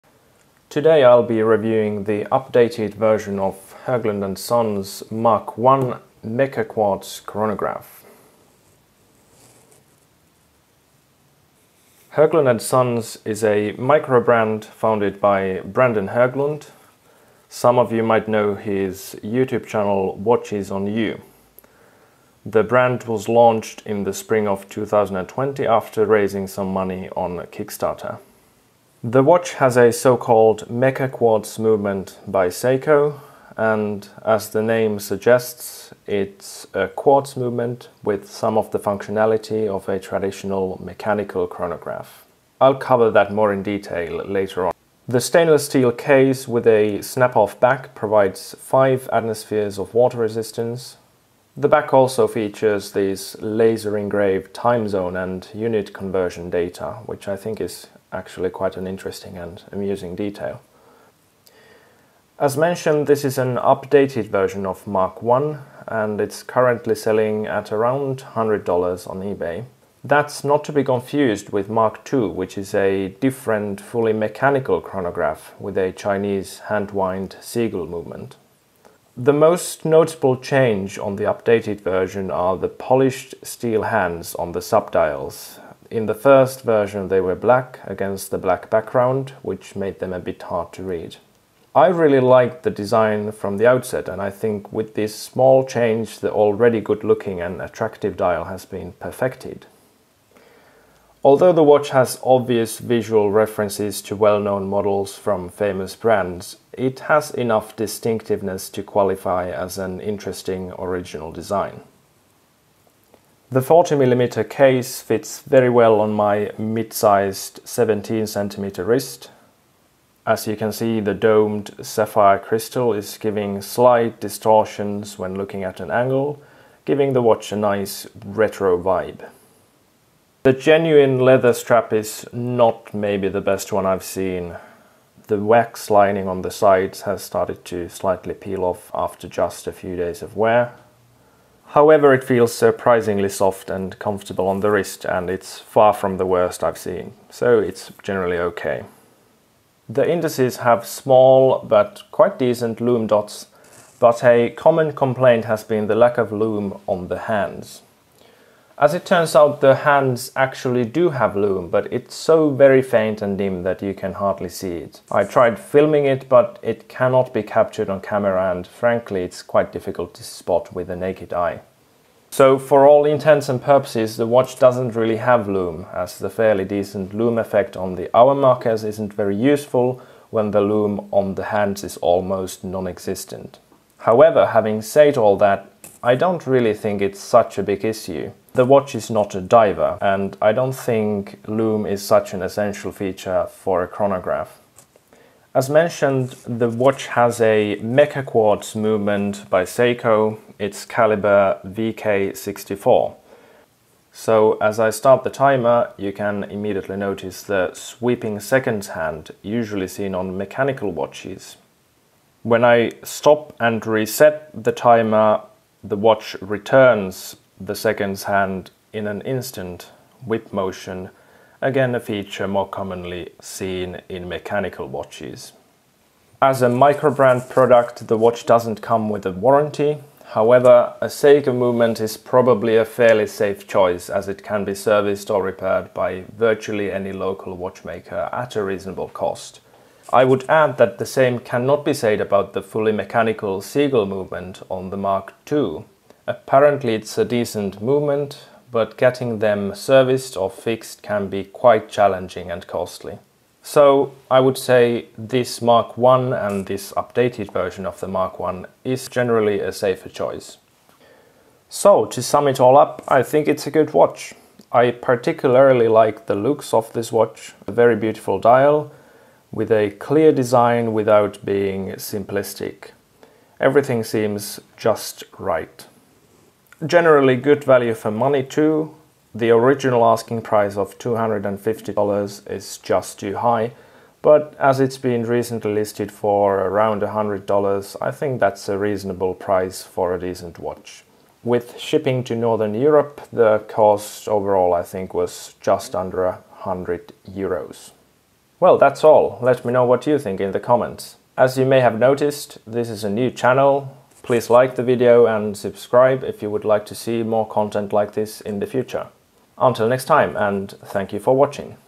Review: Höglund & Sons Mark I Mecha-Quartz Chronograph, Updated Model
Review of the updated Höglund & Sons Mark I mecha-quartz chronograph.

Mark I on Ebay